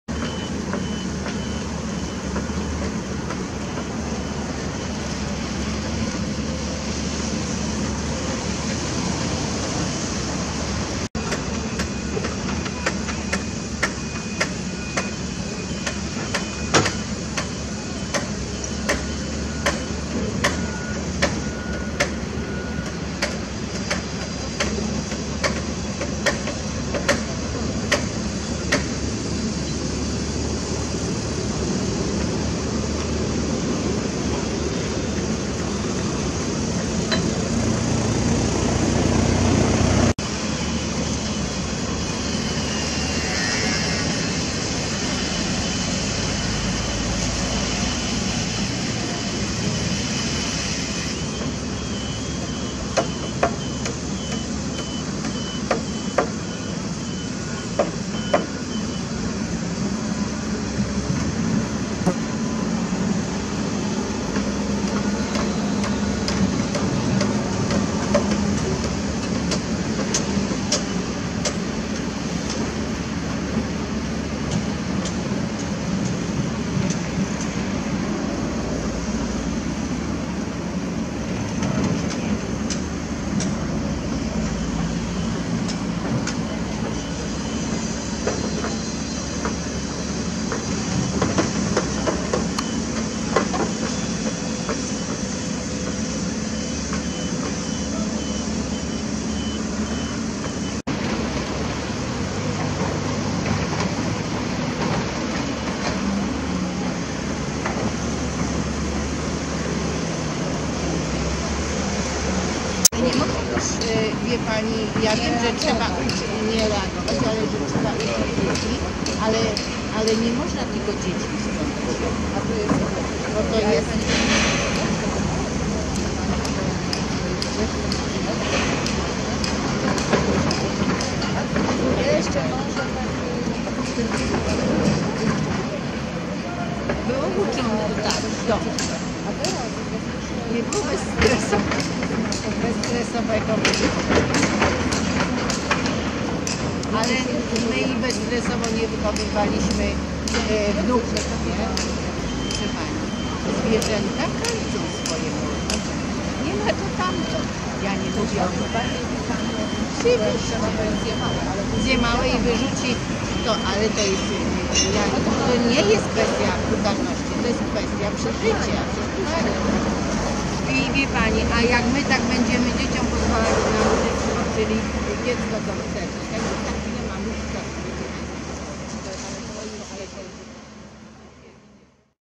Pomarańczowe rury (z archiwum przekopu Mierzei Wiślanej - 2021)
Przewożenie ładunku z czterema pomarańczowymi rurami za pomocą koparko-ładowarki JCB na budowie przekopu Mierzei Wiślanej między Skowronkami a Przebrnem. Tak to właśnie wygląda.